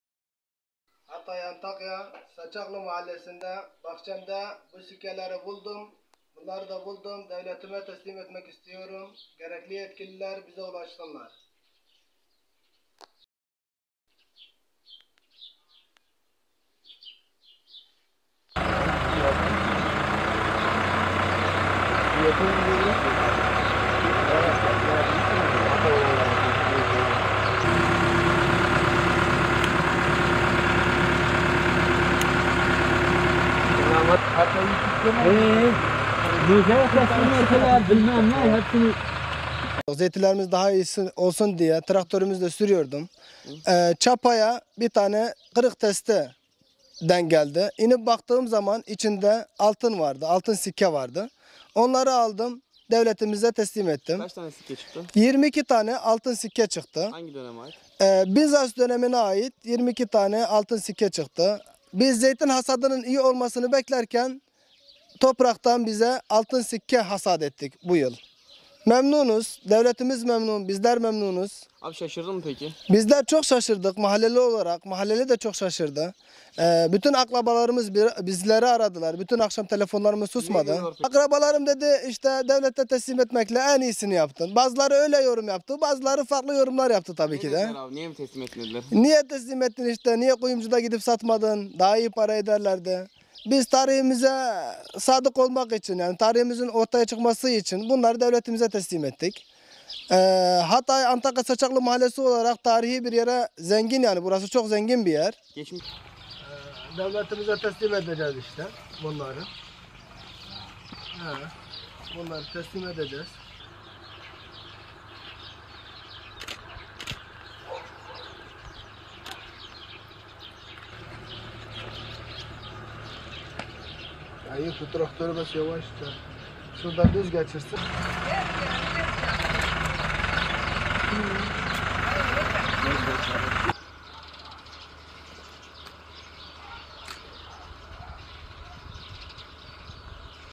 Çiftçinin pulluğuna takıldı, topraktan altın hasat etti
Hatay’da bir çiftçi, bahçesini çapalarken traktörünün pulluğuna takılan küpten çıkan Bizans dönemine ait 22 altın sikkeyi yetkililere teslim etti.